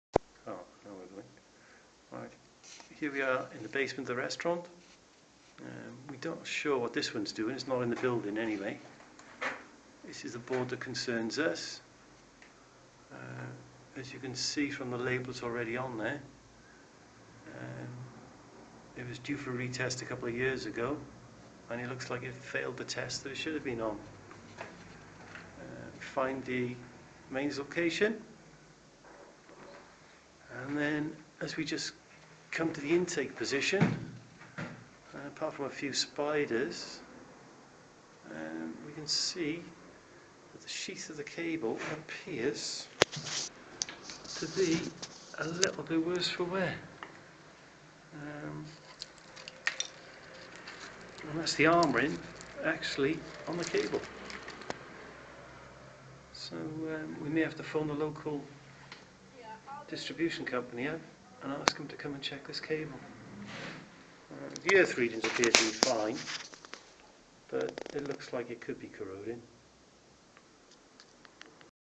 Restaurant Main intake position
Visual inspection of main incoming supply to local restaurant. We were contacted to carry out an Electrical Installation Condition Report and the first item on the form is the codition of the mains intake position.